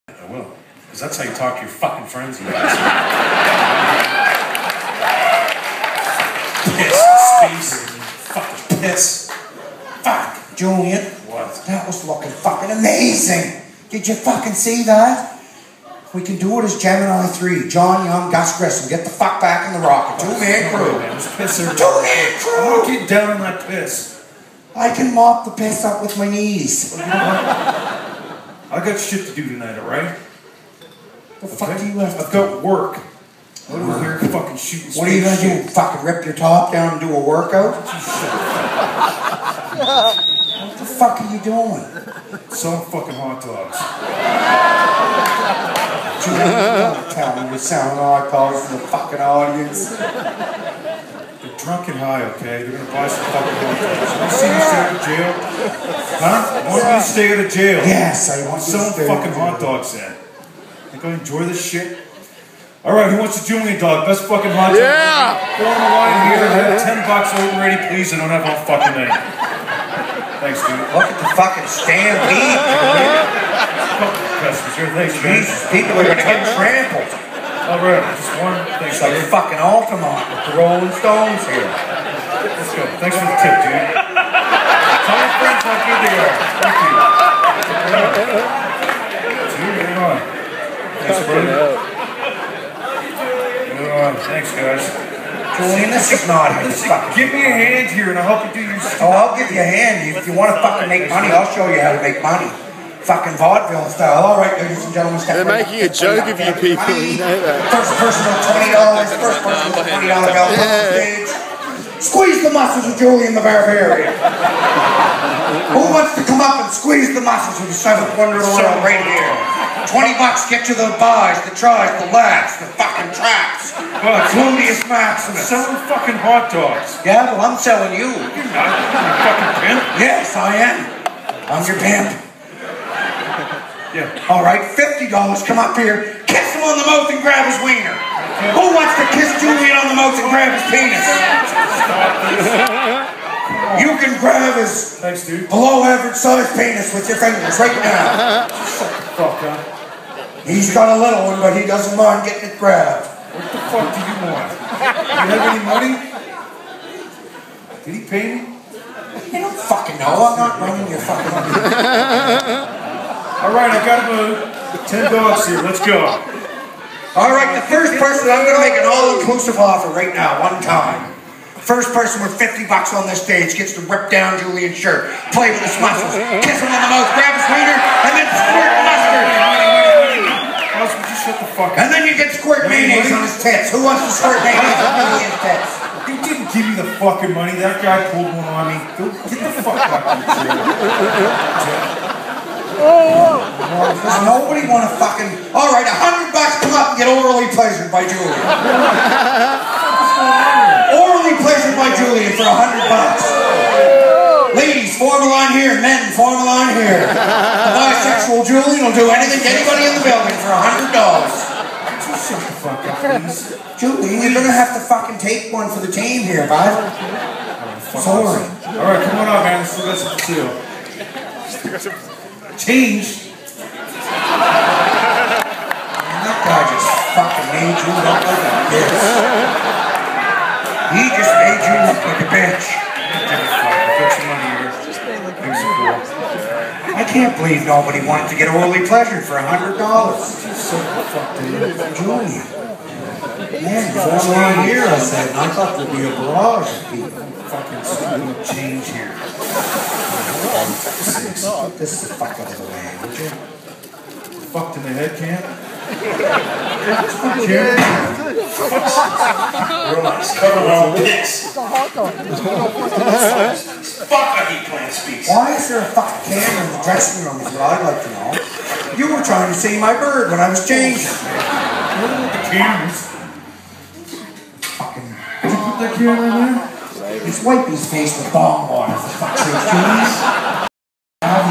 Julian (John Paul Tremblay) from Trailer Park Boys - Drunk High and Unemployed - Tivoli Brisbane
20.03.12 The Tivoli, Brisbane - Trailer Park Boys. Drunk High & Unemployed Tour, Julian selling hotdogs for $10, and many fools buying them.